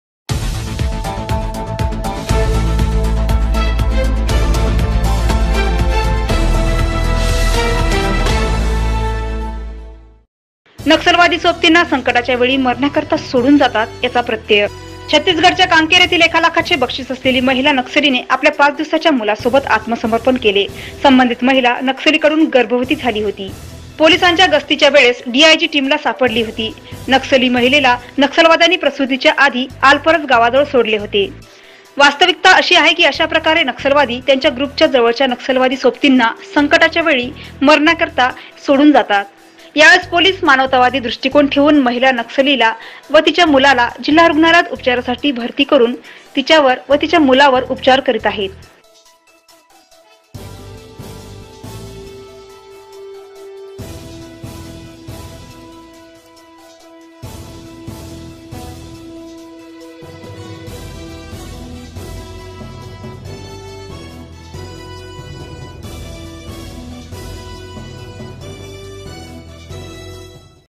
महिला नक्सली सह पाच दिवसाच्या बाळास केले उपचारासाठी दाखल...
नक्सलवादी सोबतिंना संकटाचा वेळी मरण्याकरिता सोडुन जातात याचा प्रत्यय छतीसगड च्या कांकेर येथील एक लाखांचे बक्षिस असलेली महिला नक्सली ने आपल्या पाच दिवसांचा मुलासोबत आत्मसमर्पन केले.संबधीत महिला नक्सीलीकडुन गर्भवती झाली होती.पोलीसांचा गस्ताचा वेळेस डीआजी टीम ला सापडली होती.नक्सली महिले नसलवाद्यांनी प्रसुतीचा आधी आलपरस गावाजवळ सोडले होते. वास्तविकता  अशी आहे की अशा प्रकारे
नकसलवाद्यी  त्यांचा ग्रुपचा  जवळचा नक्सलवादी सोबतिंना संकटाचा वेळी मरण्याकरिता सोडुन जातात.यावेळेस पोलीसांचा मानवतावादी दृष्टिकोन ठेवून महिला नक्सीलीला व तीचा मुलालाचा जिल्हारूग्णालयात उपचारासाठी भरती करून तीचा व तिचा मुलावर उपचार करित आहेत.